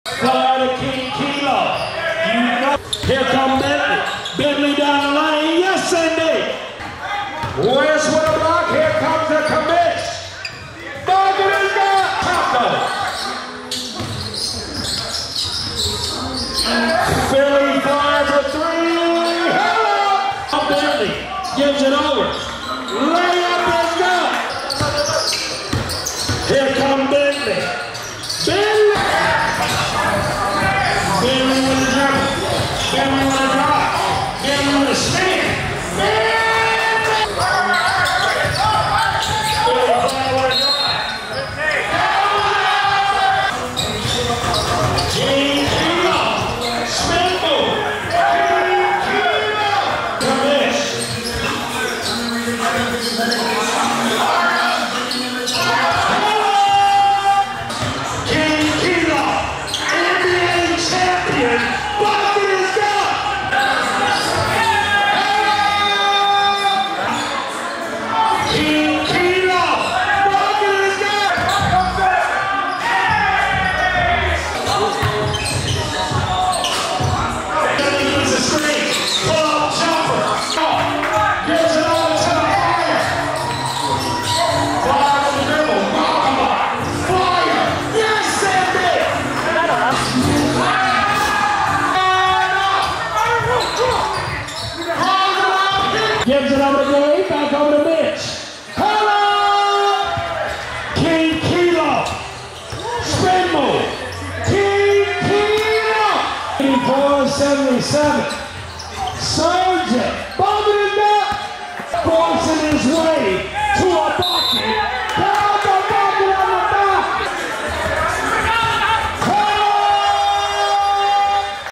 THE LEAGUE by SRGN Studios Week 10 Surgeon VS Nova
THE LEAGUE by SRGN Studios Week `10
Apologize I don't have the score outcome but I think Surgeon won.

These are only the best buckets from the game.